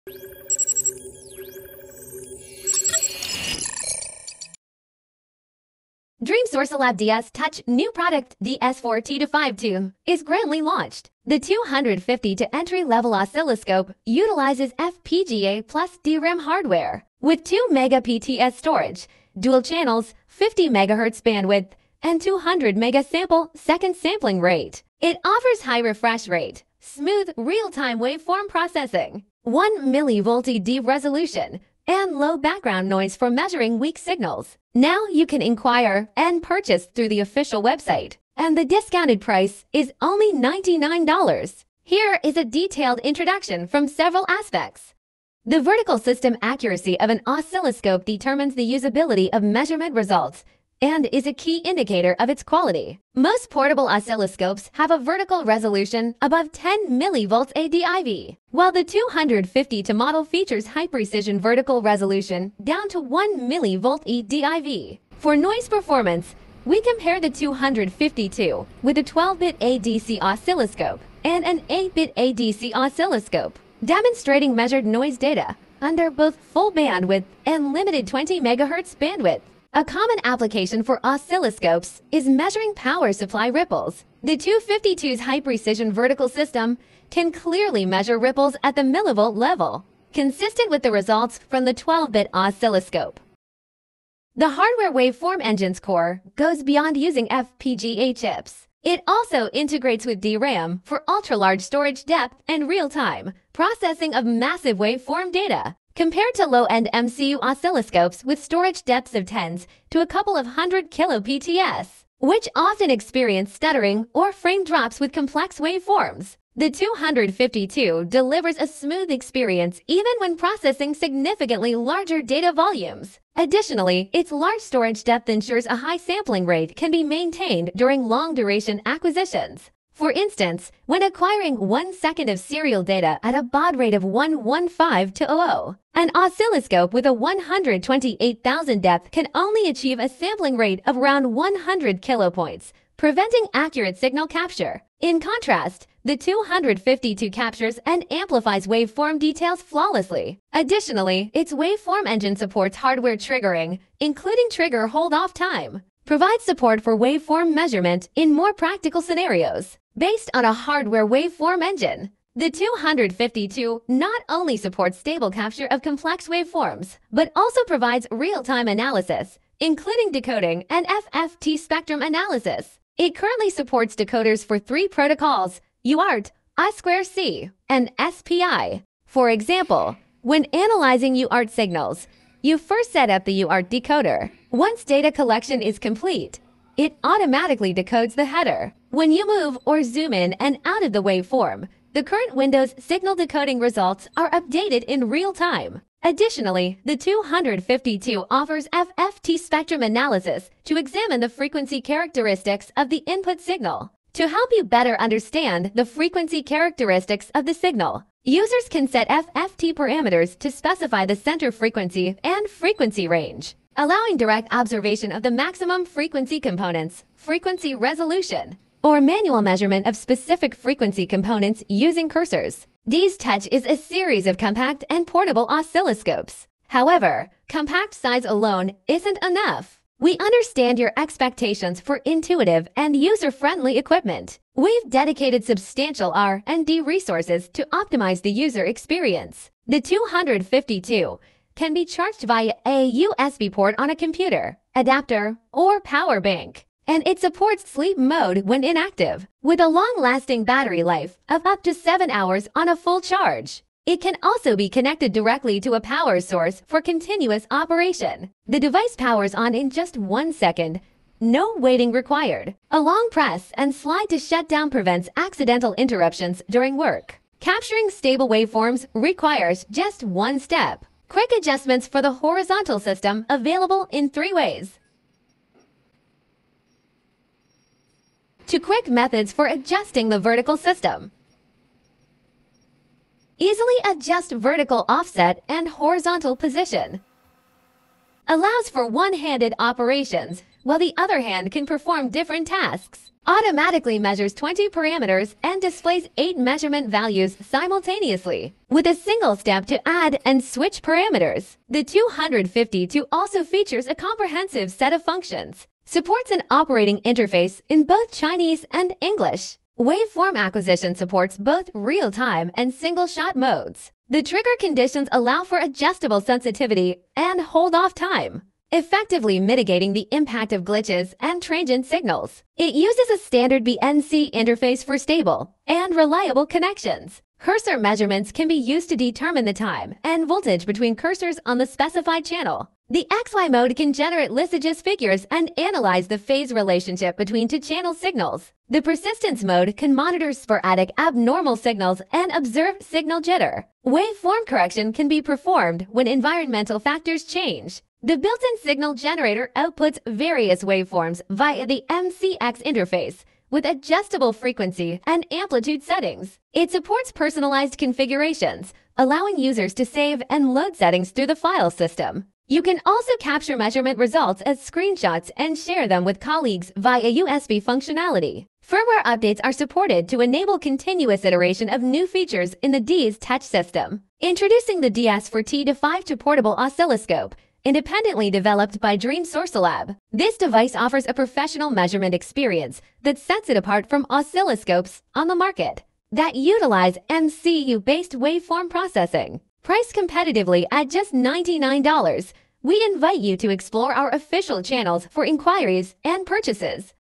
1mV/Div DSTouch Oscilloscope A New Era of Low Prices
This video introduces the advantages and features of the product.
1. Ultra-low price advantage: $99
2. 1mV/Div sensitivity
3. The oscilloscope has decoding functionality
4. Architecture: FPGA hardware waveform processing engine + DRAM storage
5. The software operation experience is convenient and fast.